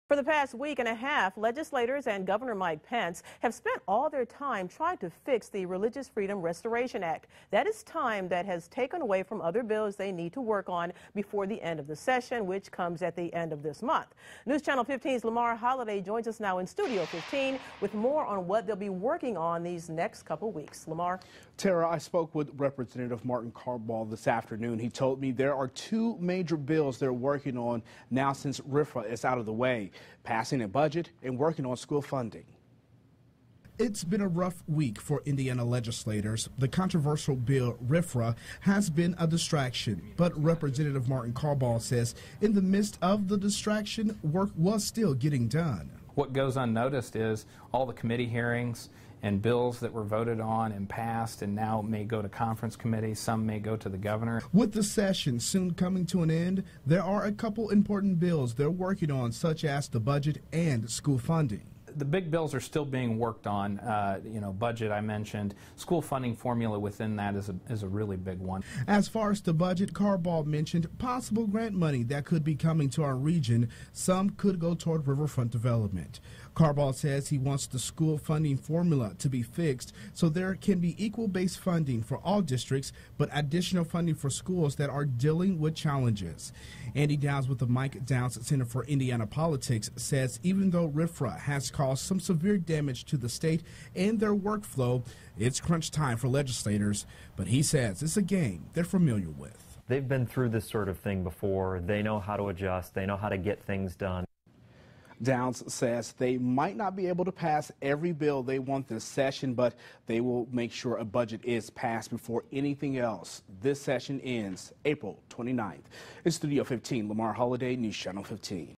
Legislators work to pass bills after RFRA debate
Indiana lawmakers are working on other major bills after dealing with the controversial Religious Freedom Restoration Act. The legislation session ends April 29.